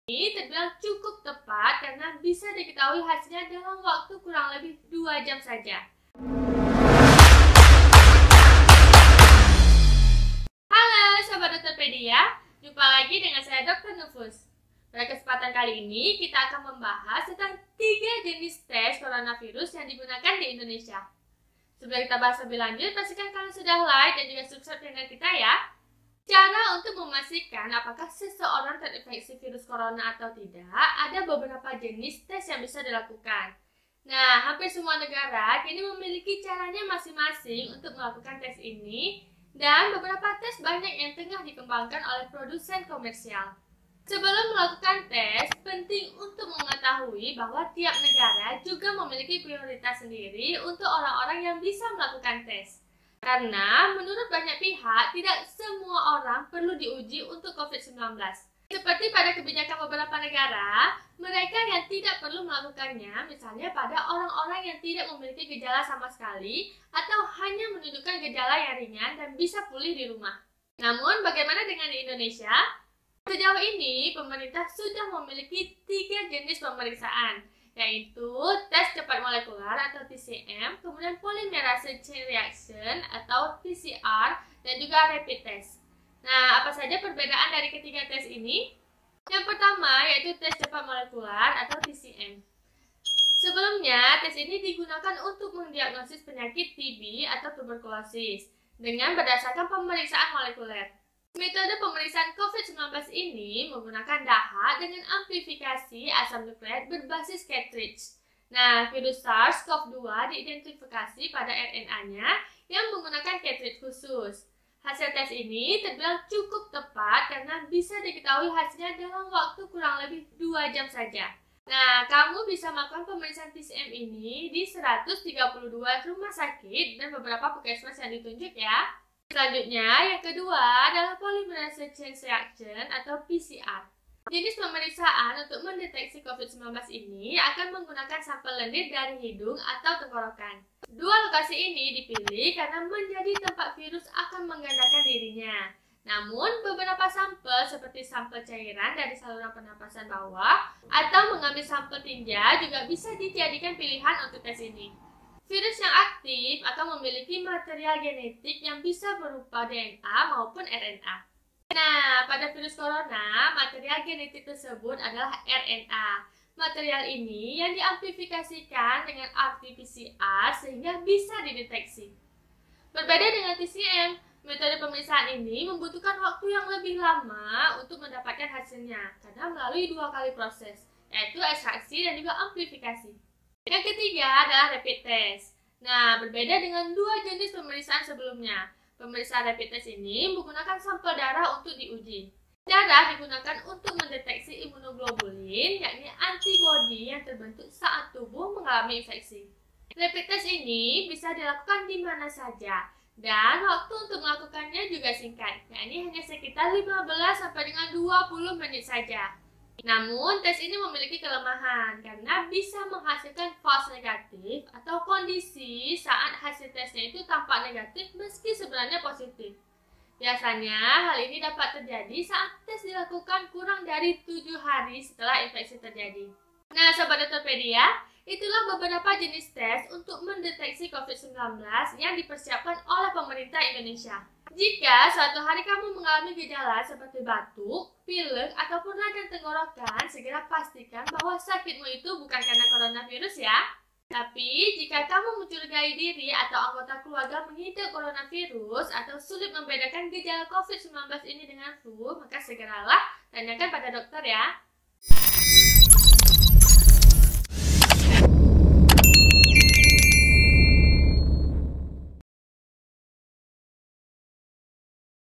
3 Cara Cek Positif Terinfeksi Virus Corona
Ada banyak pertanyaan mengenai Virus Corona atau Covid-19, Dok, bagaimana sih cara mengetahui kita positif Corona Virus? Tes nya seperti apa? dan dimana saja tes untuk mengecek terinfeksi Virus Corona atau tidak?

Nah, pada vidio kali ini kita akan membahas, 3 cara yang ada di Indonesia untuk mengetahui Positif Virus Corona, yakni tes cepat molekuler (TCM), polymerase chain reaction (PCR), dan rapid test. apa saja sih perbedaan ketiganya? Simak di vidio ya. Semoga bermanfaat...